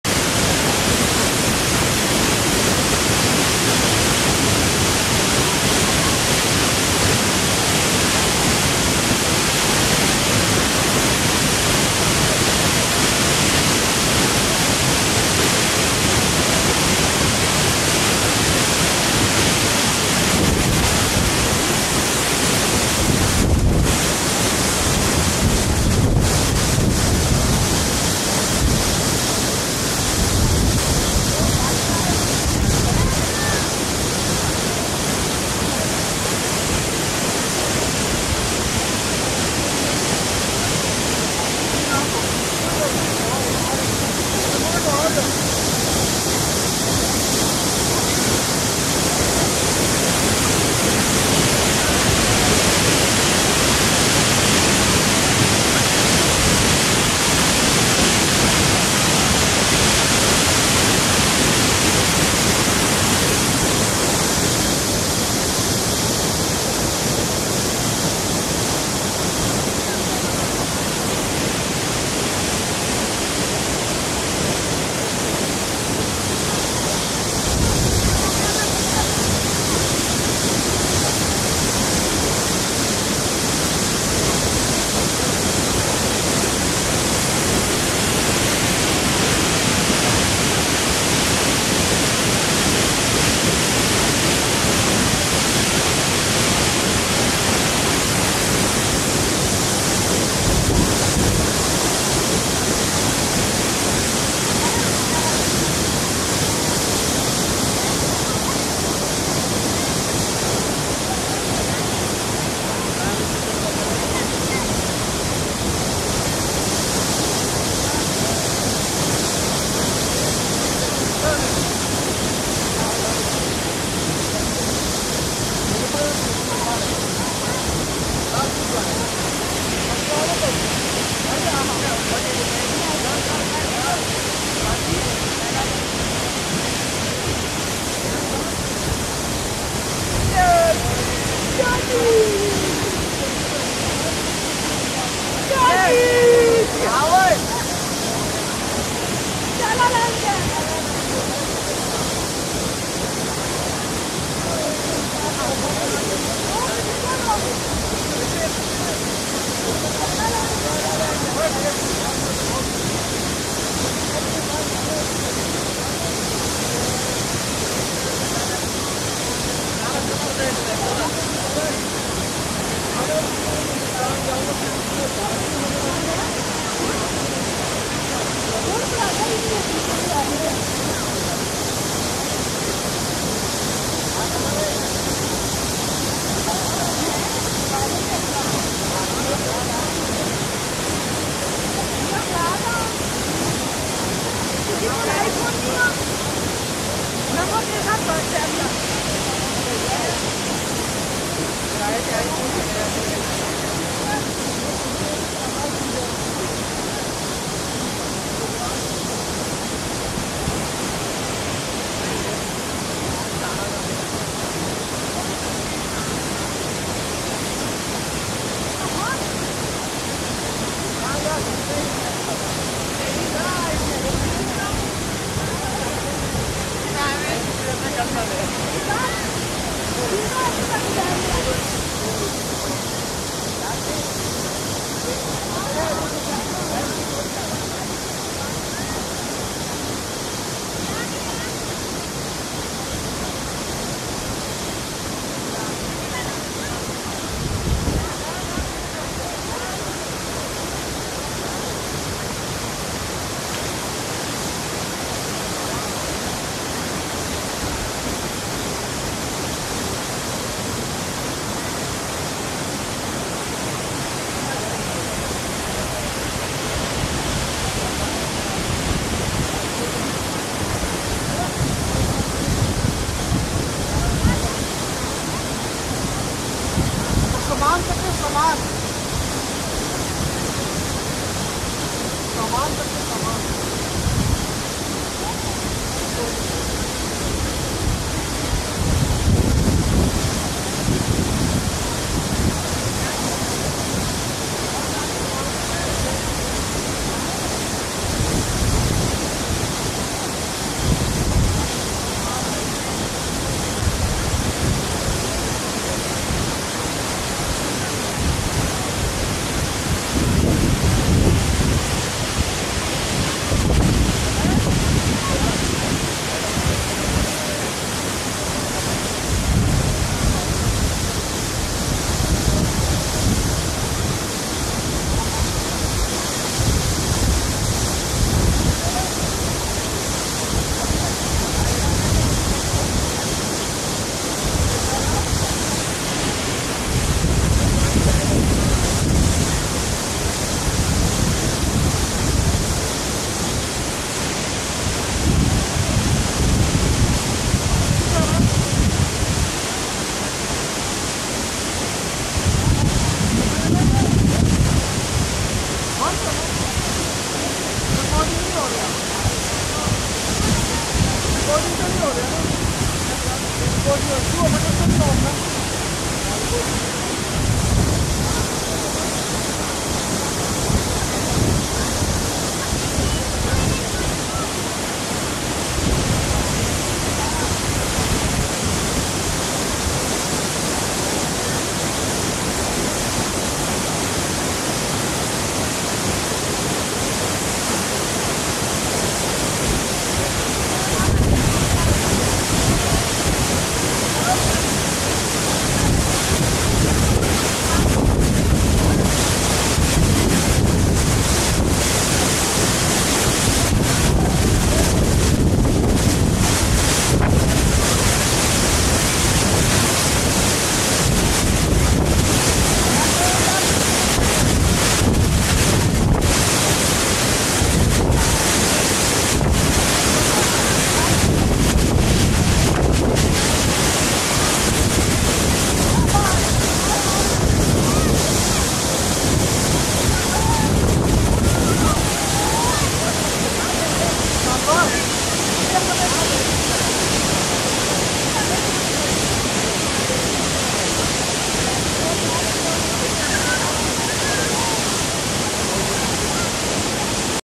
Sajikot Waterfall 360° experience - VR video of Sajikot Abshaar
You will not find any other video like this on YouTube. First time ever 360 degree video of Sajikot Waterfall.